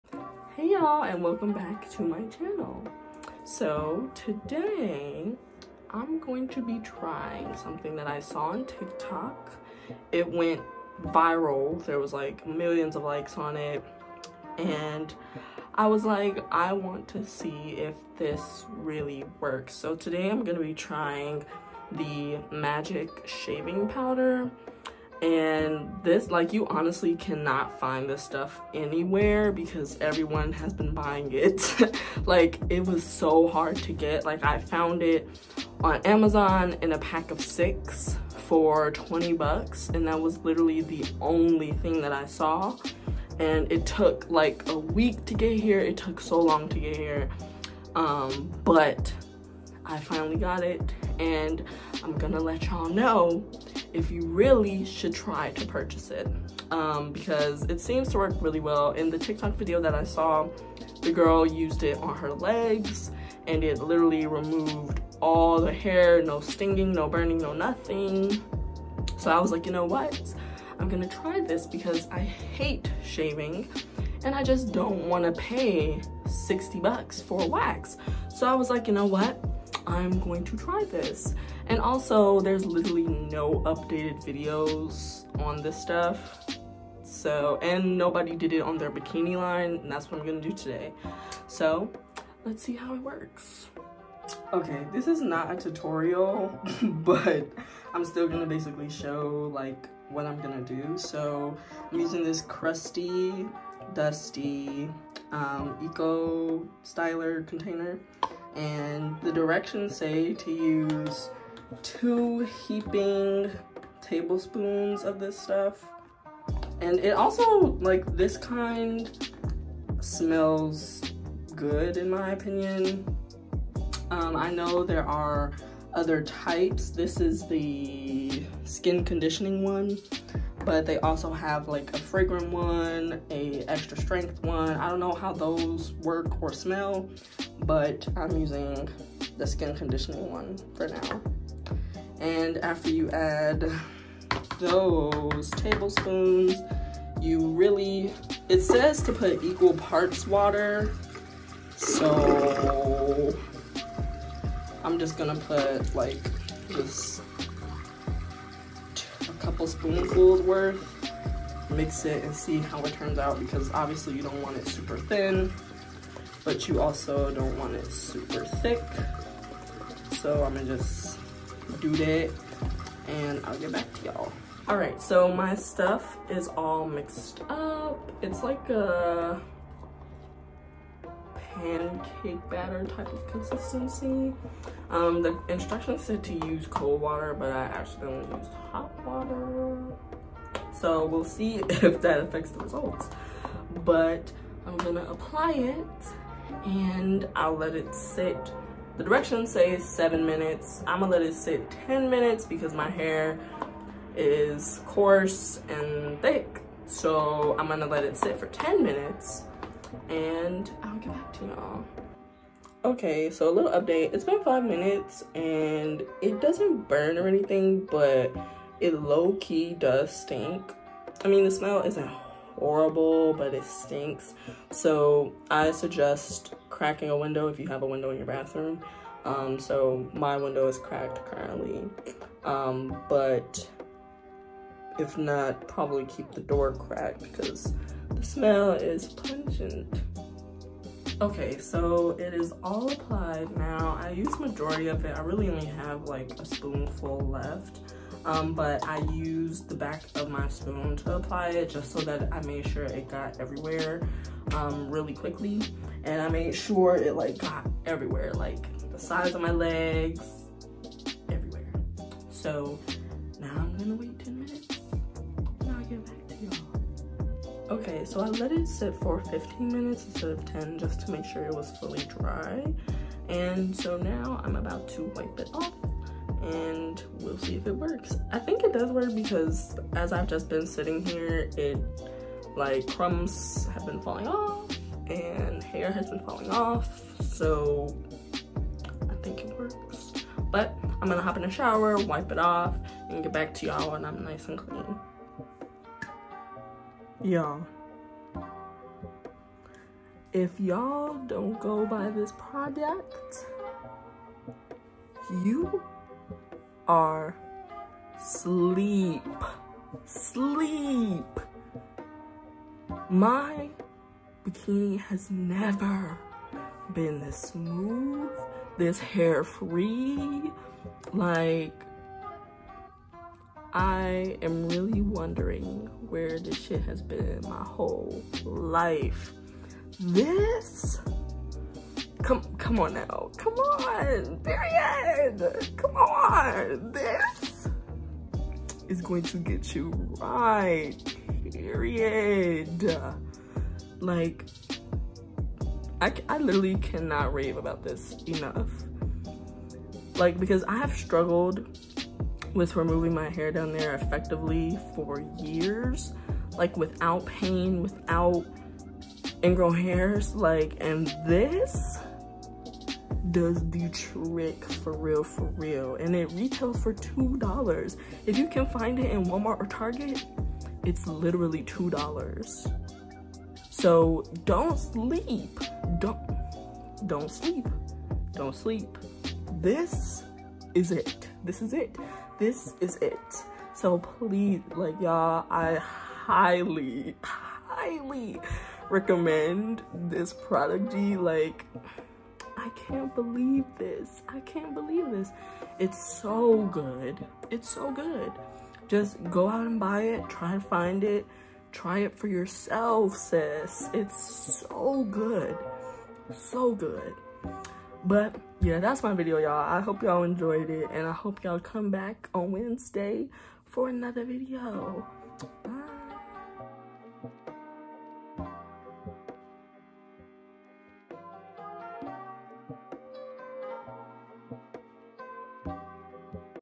TRYING MAGIC SHAVING POWDER | TIK TOK COMES THROUGH AGAIN
Hey y'all! Today, I tried a product that went VIRAL on tik tok and chile. this may be the best invention ever.